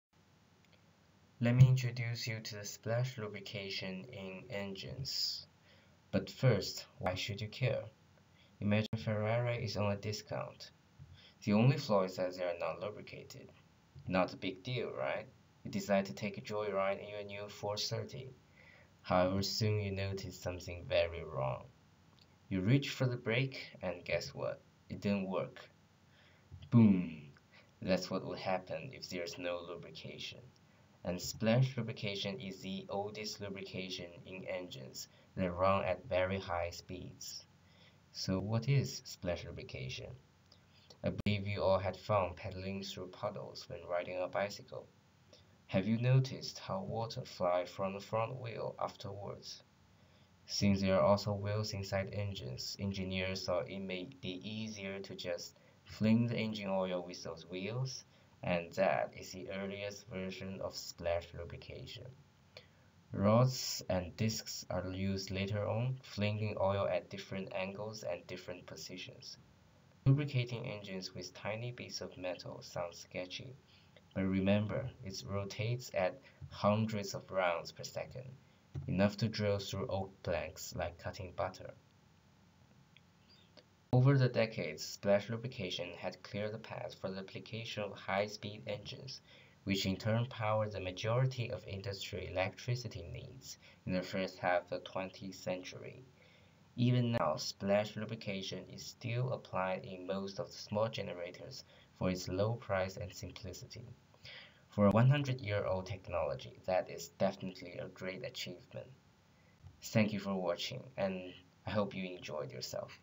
Splash Lubrication Intro
Introduction video for splash lubrication by Li Tianyi